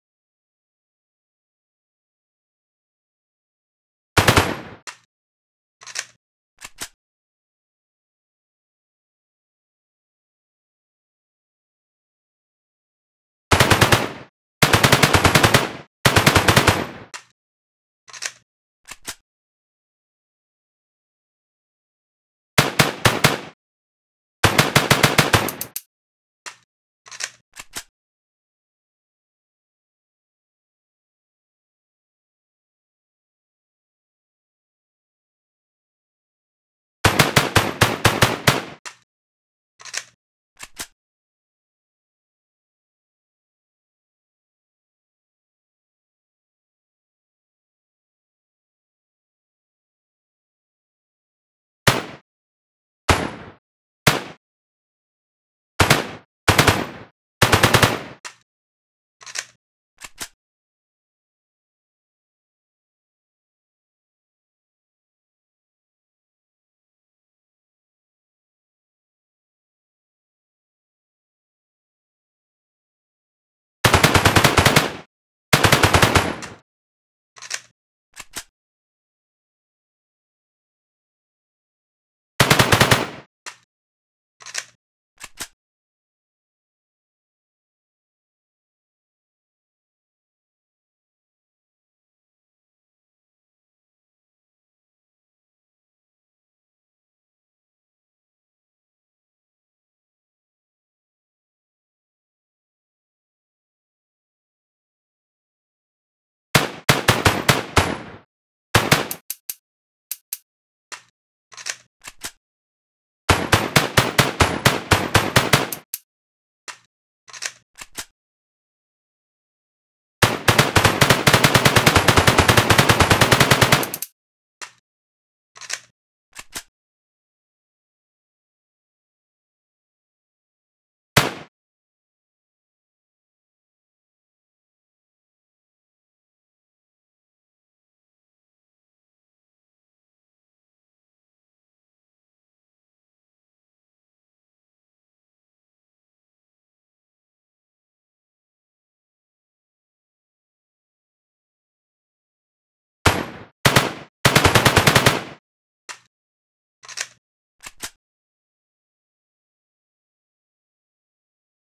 مشروع صنع لعبة!! عطوني أفكار🔥 making a new FPS game
أعتذر عن تقصيري بالمقاطع في القناة ولكنني أعمل على مشروع صنع لعبة بسبب الطفش من الألعاب الحالية.

إن شاء الله بنزل لكم مقاطع هذا الأسبوع ومن الممكن ان ينزل تحديث لباتلفيلد هذا الأسبوع🤫

مع تحيات
VIP-AHMAD-007